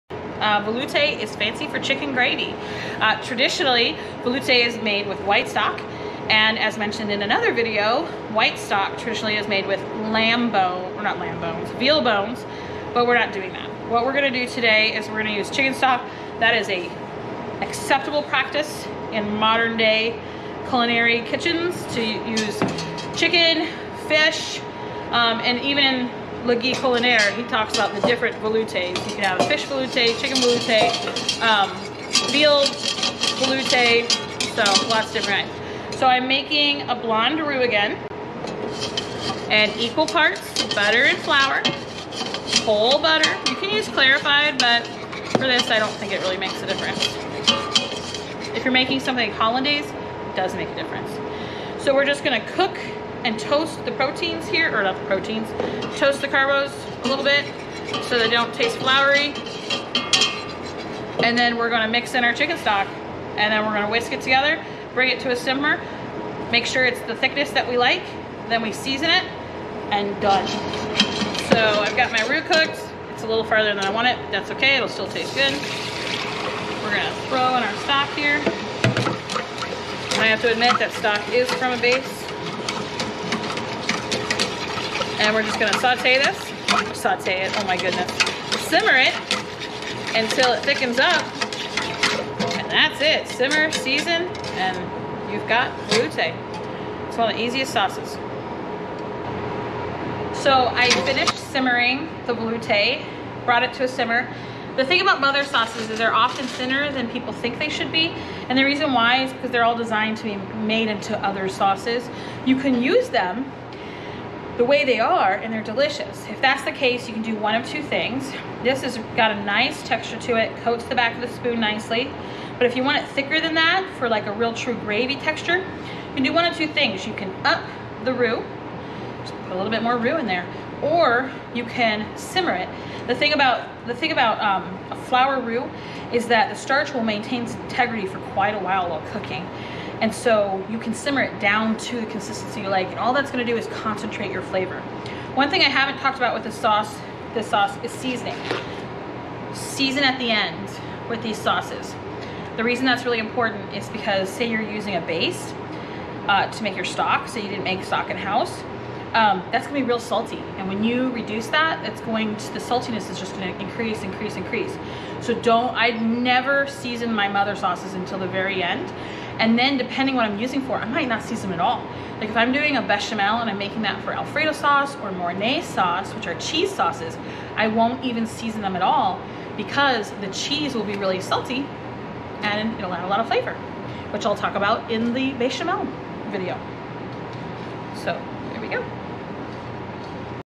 Veloute Sauce
Alden Griffus, CCC, Executive Chef at Northern Michigan University, leads us through a series of the mother sauces. This time, we learn how to make a Velouté sauce.